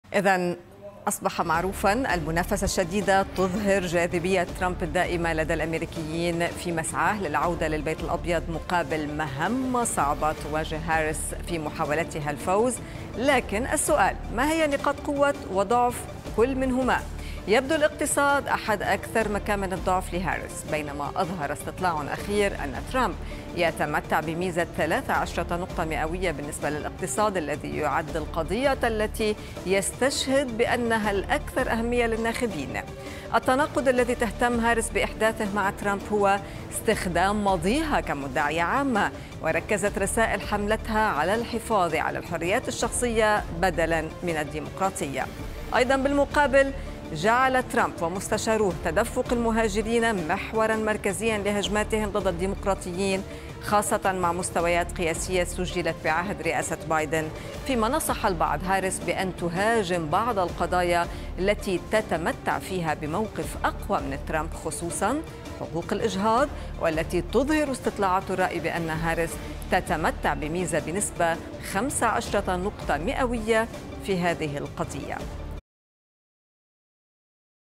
الاقتصاد أم الحريات الشخصية؟.. ترمب ينافس بالملف الأكثر شعبية وهاريس أمام مهمة صعبة
الاقتصاد أم الحريات الشخصية؟.. ترمب يجذب الأميركيين بالملف الأكثر شعبية و هاريس تواجه مهمة صعبة لمنعه من العودة إلى البيت الأبيض
لمعرفة أحدث المستجدات.. عبر البث المباشر من هنا 👇
وبإمكانكم متابعة العربية على منصات التواصل